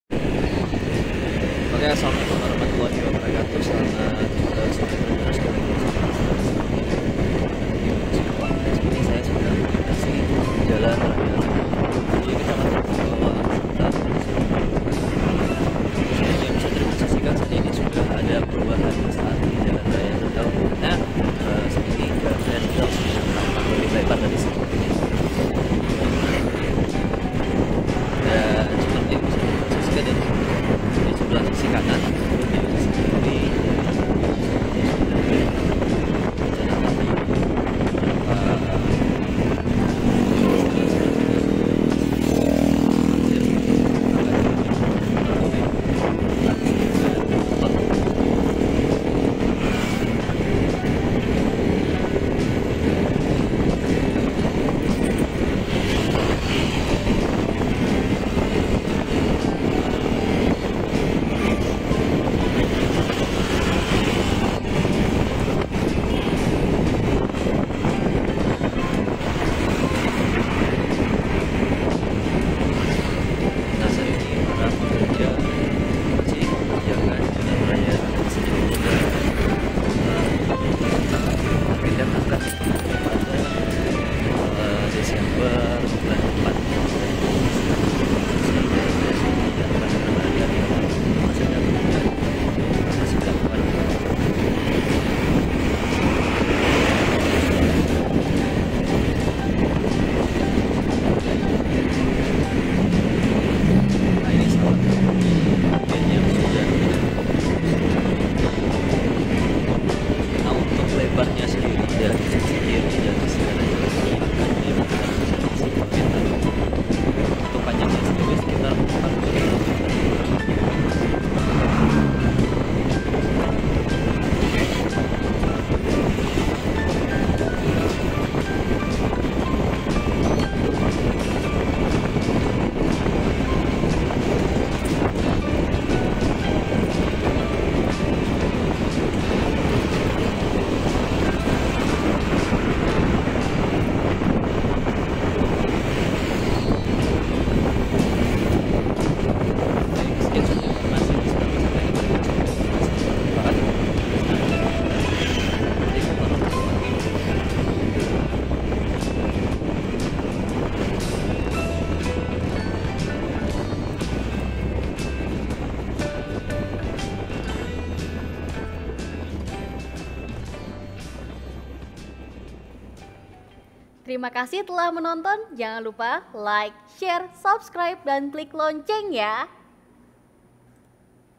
Jalan Raya Sedau di Kota Singkawang Tampak Mulai Lebar
Bayar Rp 5 Juta Ternyata yang Dibeli Tiket Hari Pertama